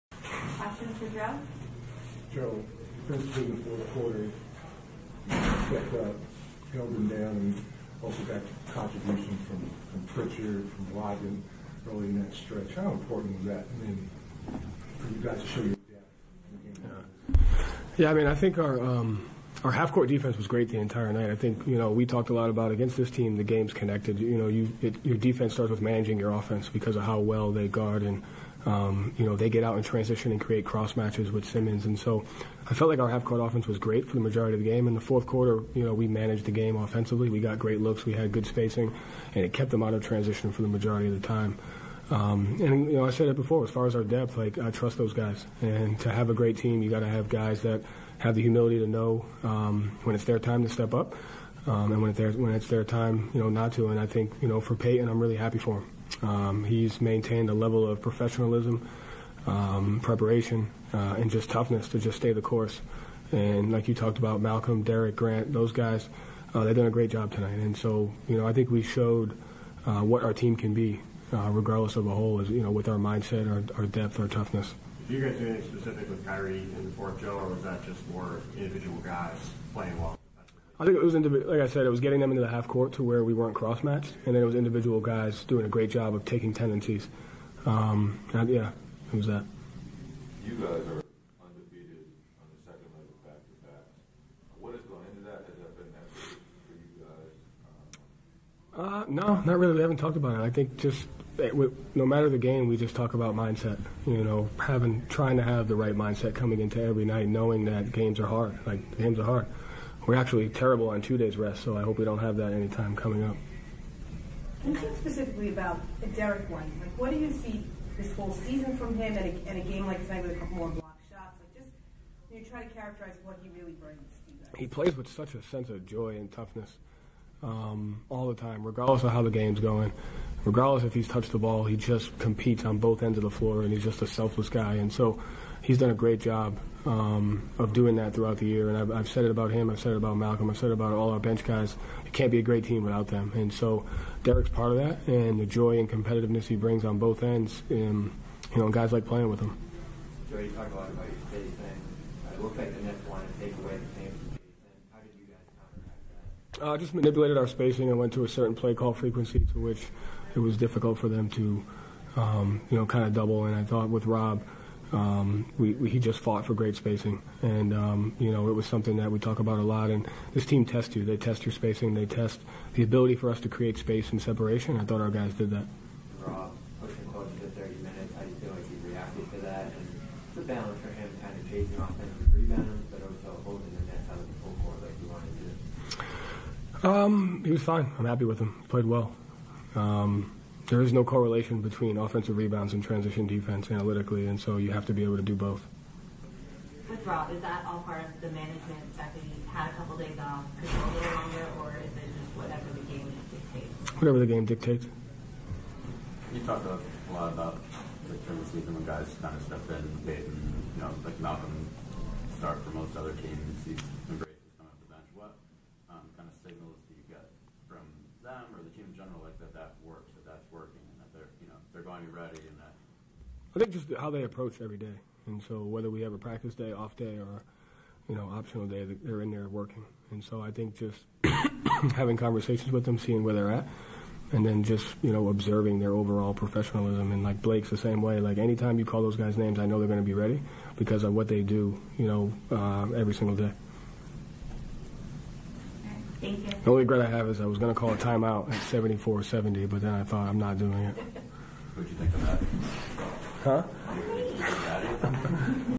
Joe Mazzulla Postgame Interview | Celtics vs Nets
BROOKLYN, NY -- Boston Celtics Interim Head Coach Joe Mazzulla was interviewed following Boston's 109-98 win over the Brooklyn Nets on Thursday night. It was the Celtics fifth straight win, and their ninth straight win against the Brooklyn Nets dating back to their first round series during the 2022 Playoffs. While Brooklyn was without superstar Kevin Durant, both Jaylen Brown and Al Horford were out for the matchup.

On the Celtics rotation players stepping up off the bench, Mazzulla said "I Trust Those Guys."